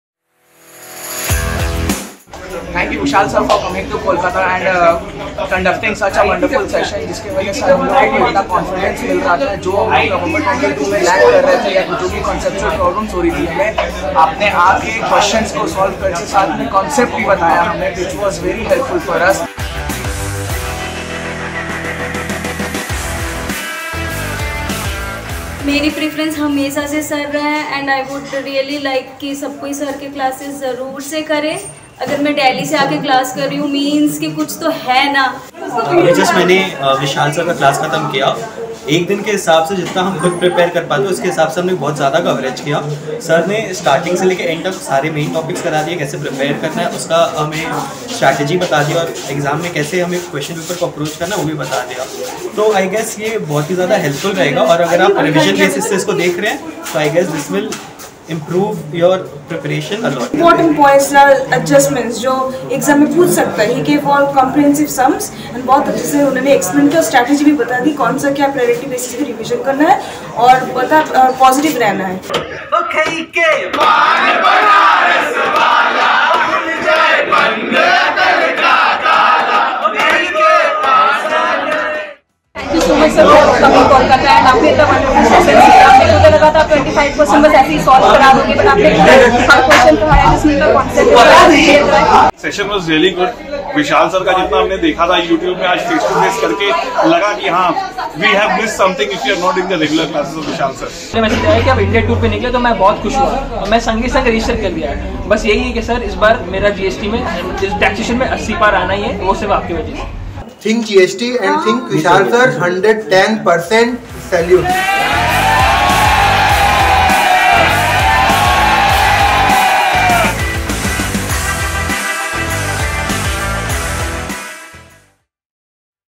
Student Voices Matter! Face to face learning experience with CA Vishal Bhattad Sir
Student Voices Matter! Check out what our students have to say about their Face to face learning experience with CA Vishal Bhattad Sir.